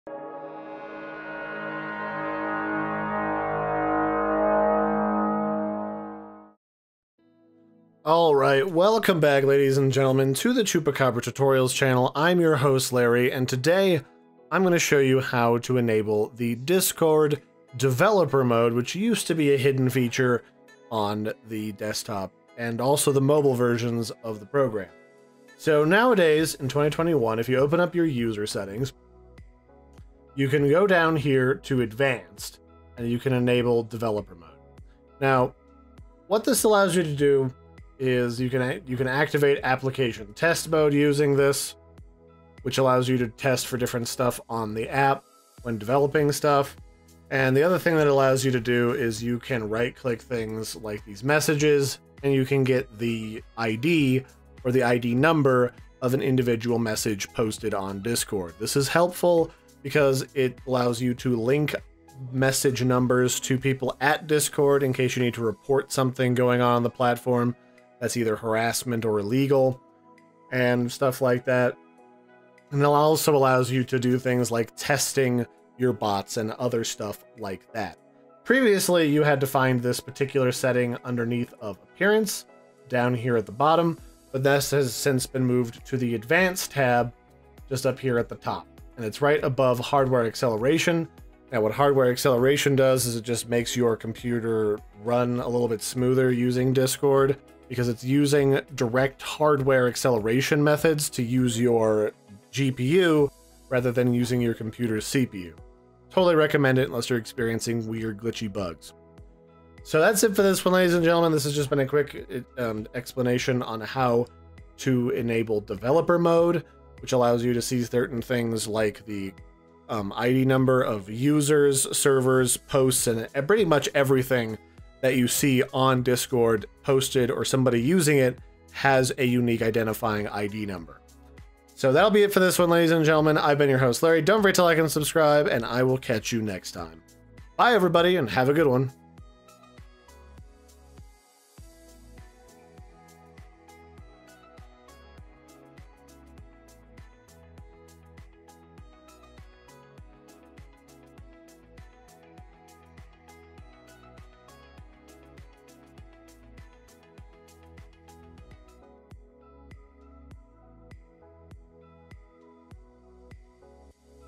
How to Enable Discord DEVELOPER Mode - User Settings Tutorial
How to Enable Discord Developer Mode on Both Desktop & Mobile. Allowing you access to the ID Number of posts, Servers, Discord Users, and other advanced features used to develop Apps & Bots.